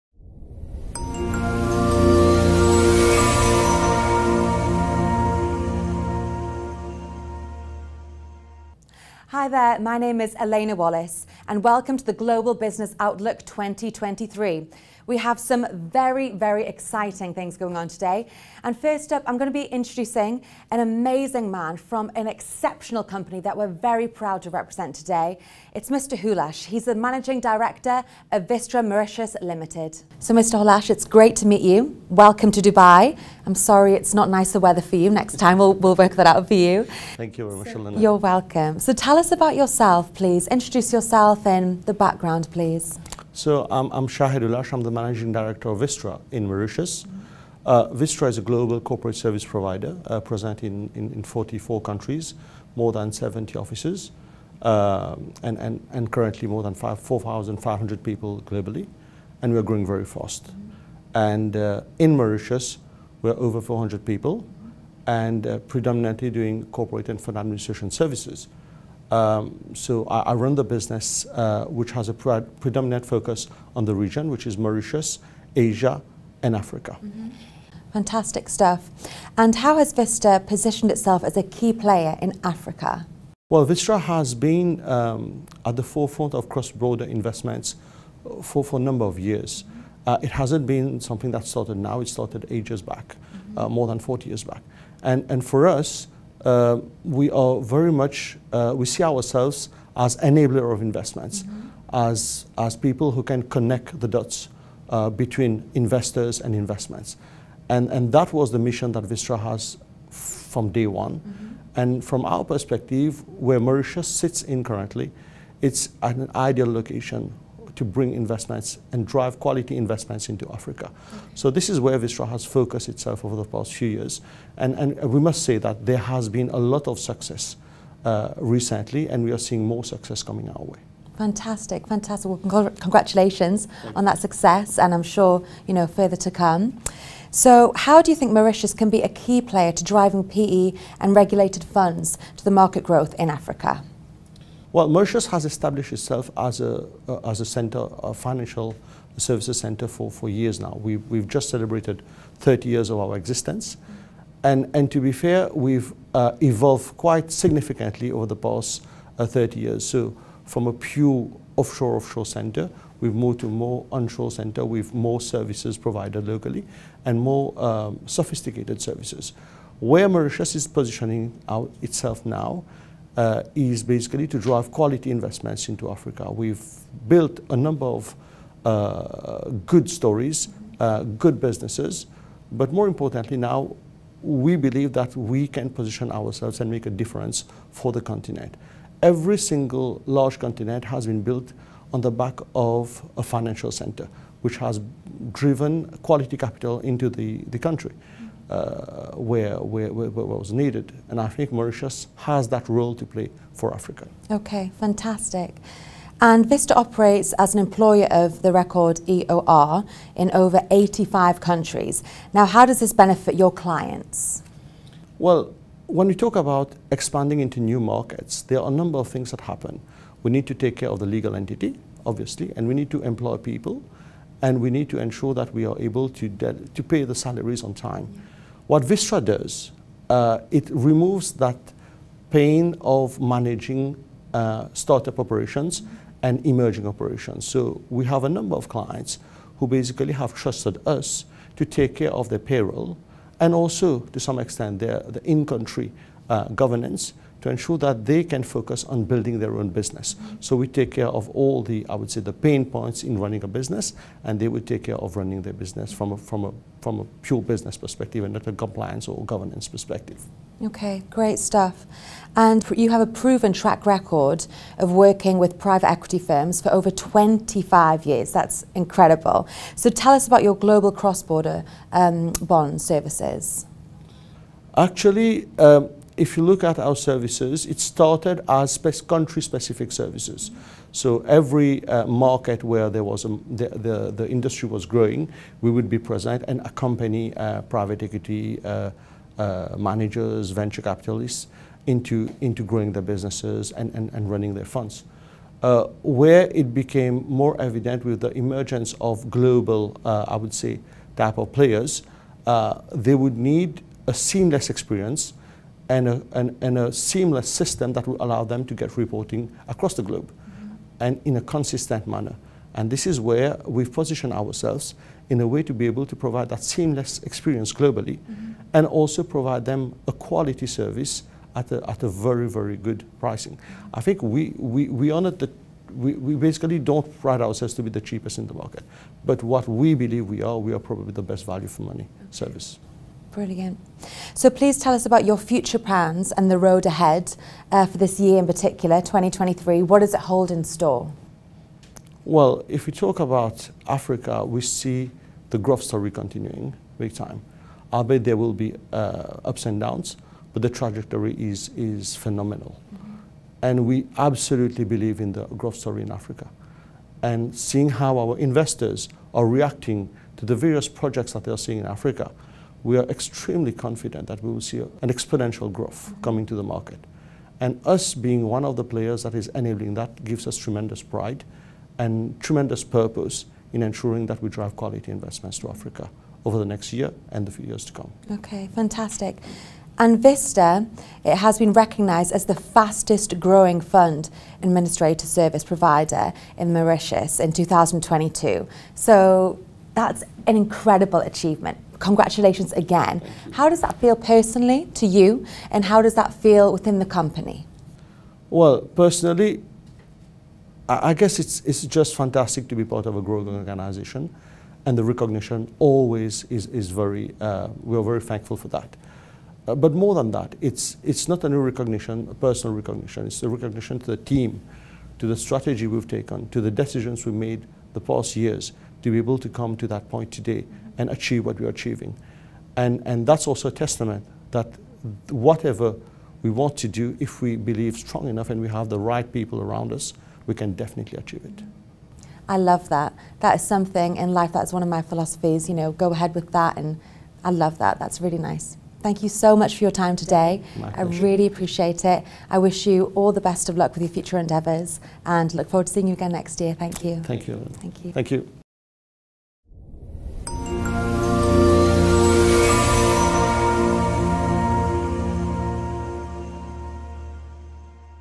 Interview with Mr. Shahed Hoolash, Managing Director of Vistra Mauritius Limited at GBO Awards 2022
Vistra is a Mauritius-based company specialising in providing corporate solutions and facilitating cross-border investments. It has over 70 offices and a dedicated team of 4500 professionals and operates in 44 countries.

The 30-year-old company, led by its Managing Director Mr. Shaheed Hoolash, focuses on the areas of Mauritius, Asia and Africa. The company was recognized by the GBO as the “Fastest Growing Fund Administrator Services Provider in Mauritius” for the year 2022.

During the occasion, Mr. Shaheed Hoolash interacted with the Global Business Outlook and spoke about the prospects of his company and Mauritius being key players in driving PE and regulated funds to fuel Africa's market growth, Vistra’s Employer of Record (EOR), its cloud-based global payroll and other cross-border bond mechanisms, plans and roadmap for the near future.

Mr. Shaheed Hoolash also gave his insights about the role his company plays in ensuring Mauritius’ clean status on the tax front.